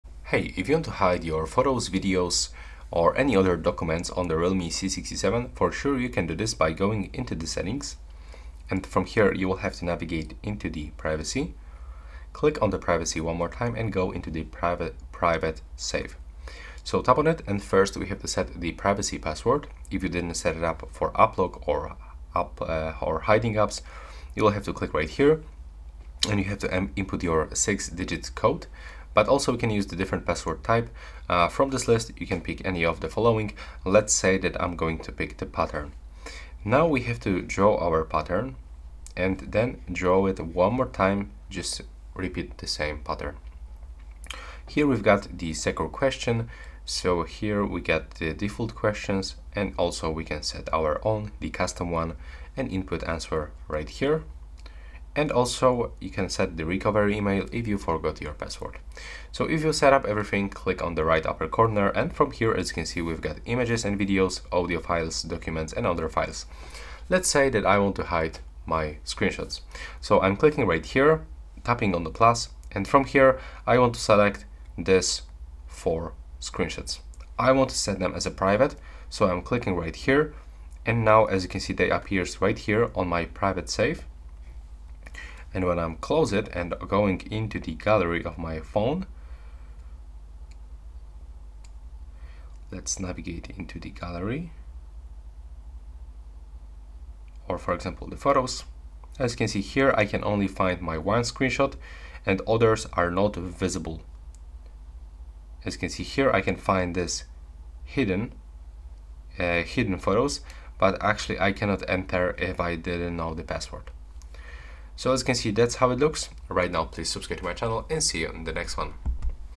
Realme C67: Hide Photos, Videos & Documents - Enable Private Safe
Safeguard your personal content on the Realme C67 by learning how to hide photos, videos, and documents and enable Private Safe with our tutorial video. Follow the steps to seamlessly enhance the privacy of your files, ensuring they remain secure and confidential on your Realme smartphone.